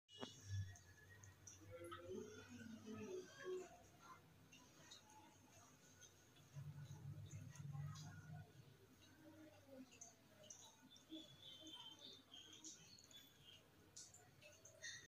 Super Kitten (Cuteness Overload)
kitty clean well after a good sleep